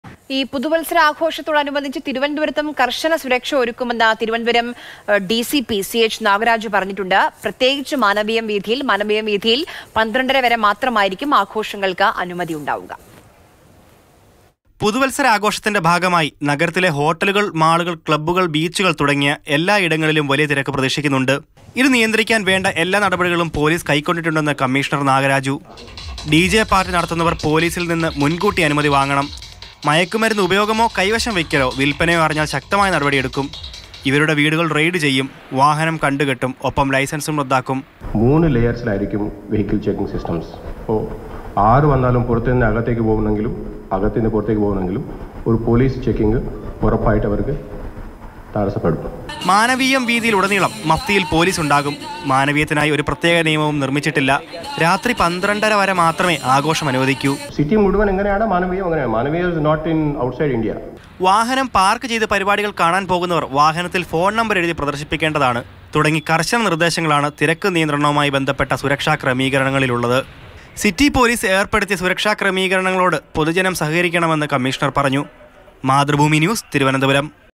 DJ പാർട്ടി നടത്തുന്നവർ മുൻകൂട്ടി അനുമതി വാങ്ങണം; പുതുവത്സരാഘോഷത്തിന് കർശന സുരക്ഷ ഒരുക്കാൻ പോലീസ്
പുതുവത്സരാഘോഷത്തിനൊരുങ്ങി തലസ്ഥാനം; കർശന സുരക്ഷ ഒരുക്കാൻ പോലീസ്

Happy viewing!